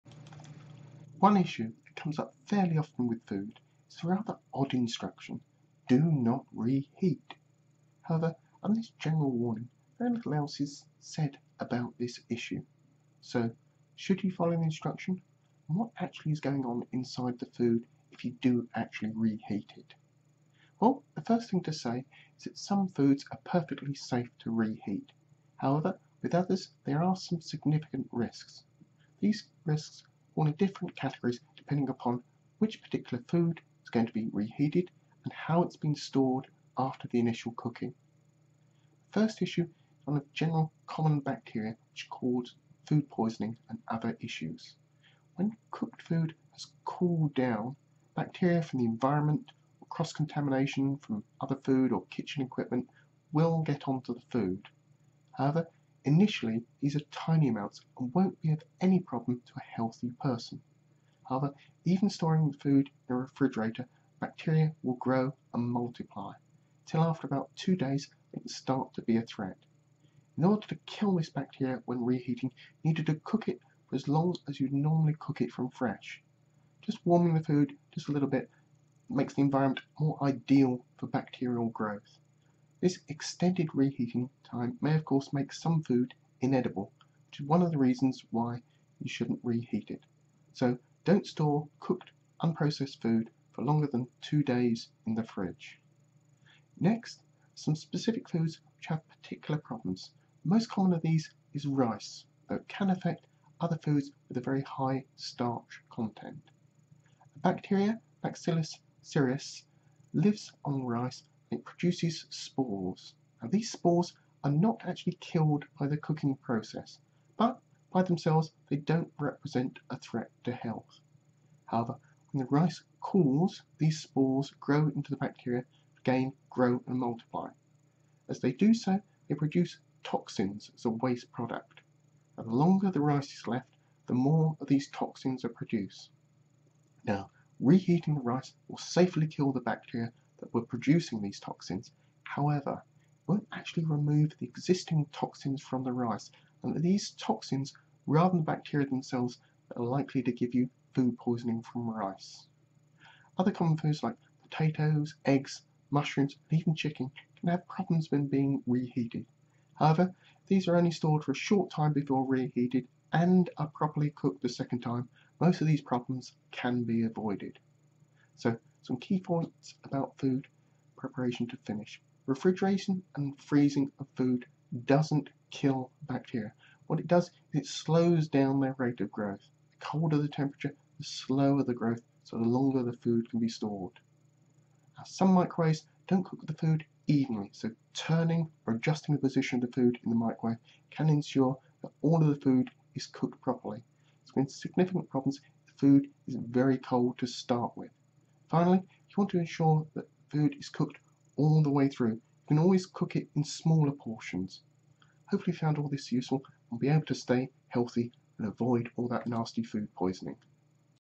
Reheating food, what are some of the risks?
A look at refrigeration and storage of cooked food and the action of the bacteria Bacillus Cereus which can be on rice and produce toxins which cause problems.